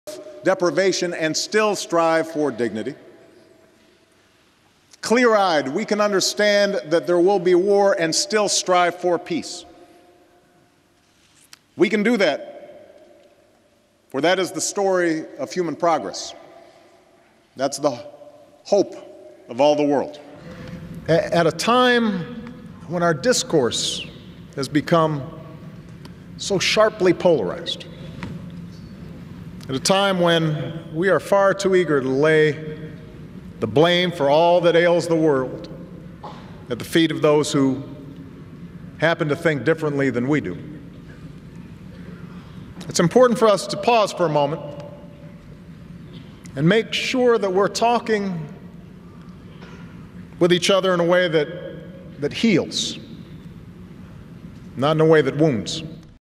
5/10/18 Obama Excerpts
I try not to be political but watching the news this week I think it's time to say somethings.  The white roses are for PEACE.